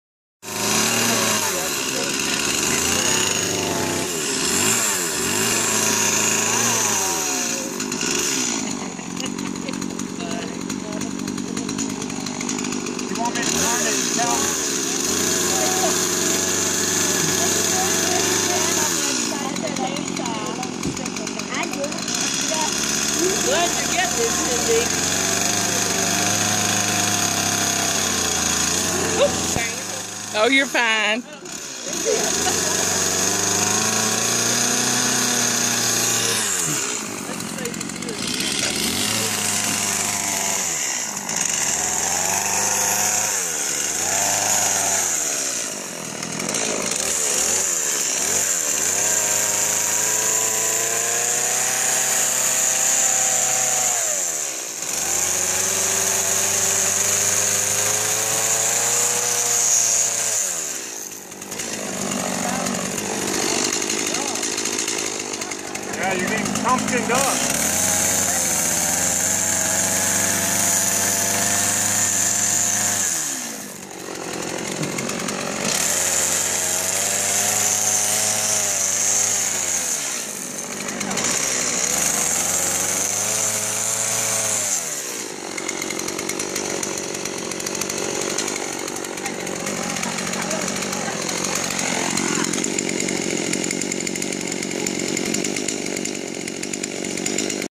Clyde Jones + chain saw + pumpkin = Jack o'lantern!
Famous North Carolina chain saw artist Clyde Jones "tears one up" with a chain saw. Bynum, North Carolina is known for their fantastic display of Jack o'lanterns on Halloween night.